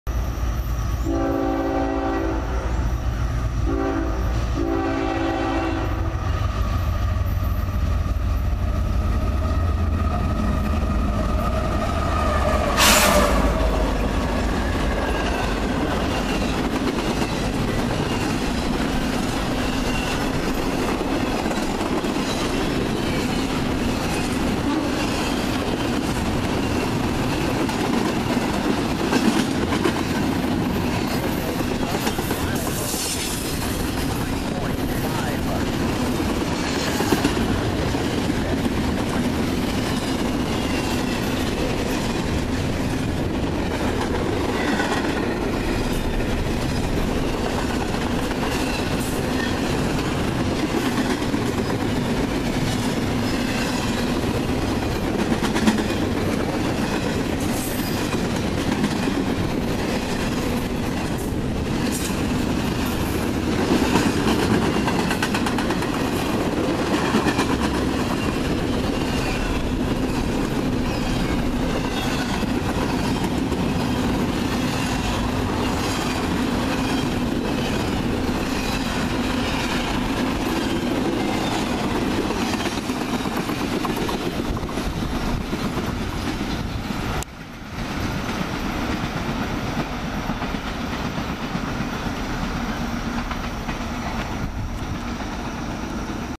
Westbound intermodal with UP 7377 as the leader
This train has a different scene compared to the last one.  This one looms out of a nice sunrise and it's been awhile since I saw a Union Pacific locomotive leading a train in this area.  I hope you like this one.